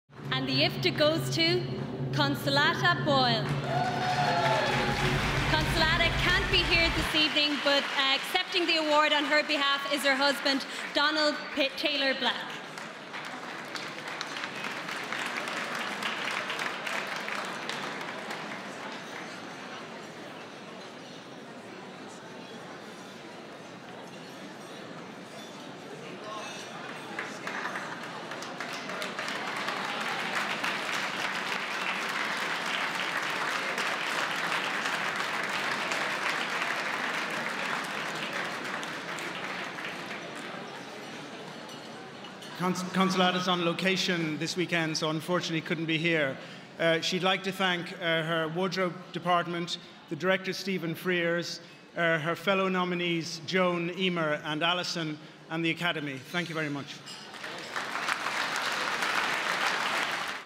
Consolata Boyle, IFTA Winner 2010, Costume Design for Cheri
Donald Taylor Black accepts the IFTA for Costume Design on behalf of his wife Consolata Boyle for her work on Cheri at the 7th Annual Irish Film & Television Awards in Dublin on 20 February 2010